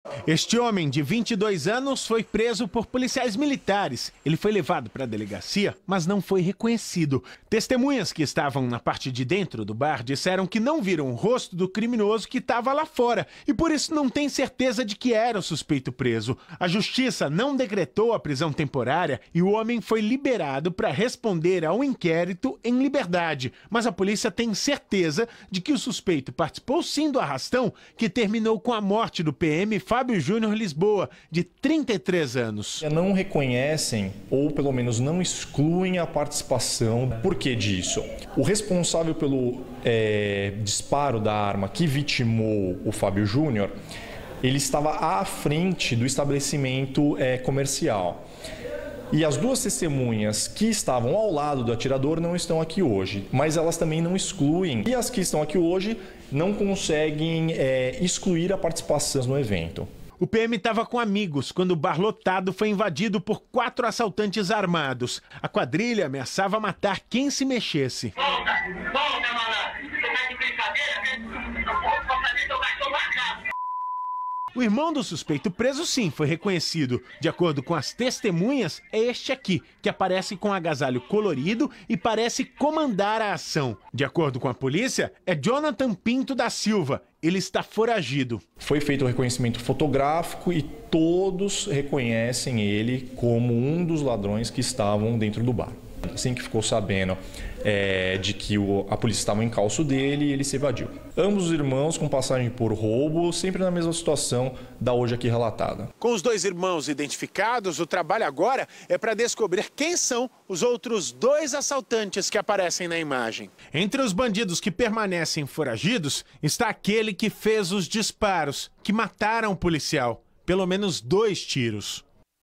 Justiça manda soltar suspeito de matar PM em bar
Polícia prende, mas Justiça manda soltar suspeito de matar policial dentro de um bar.